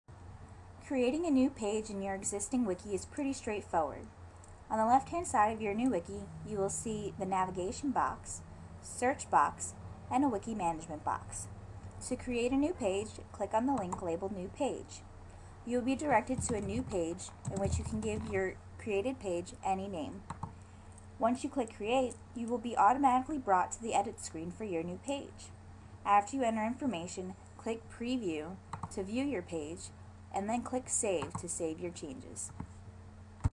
How to Create a New Page in Wikischolars
This tutorial explains how to create a new Wikischolars page. A brief walk through of the process of creating a page. Naming and creating of page are explored.